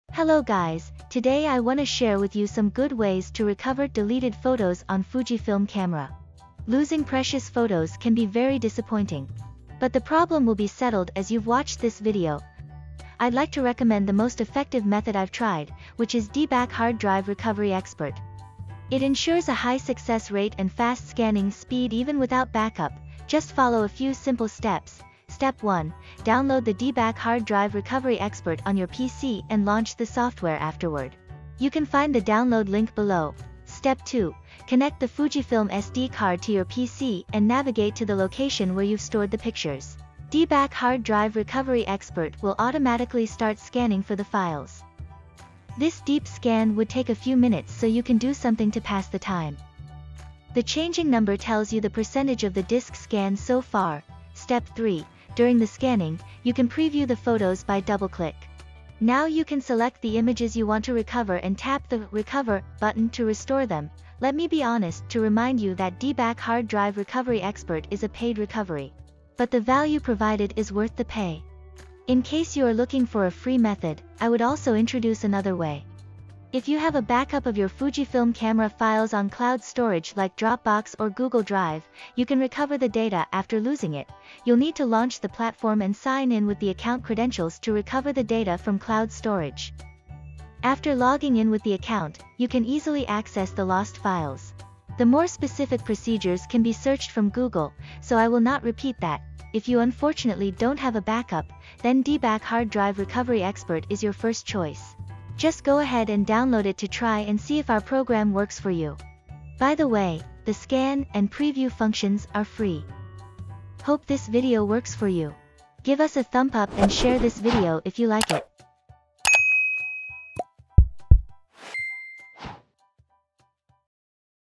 How to Recover Deleted Photos on Fujifilm Camera?
If you're troubled by losing precious photos on Fujifilm Camera, watch this video to find the most efficient and easiest way to recover them.
👉 Timestamps:
👉00: 13  Method 1: Recover Deleted Photos with D-Back Hard Drive Recovery Expert
👉01: 12  Method 2: Recover Deleted Photos via Dropbox or Google Drive with Backup
👉01: 38  Recover Deleted Photos without Backup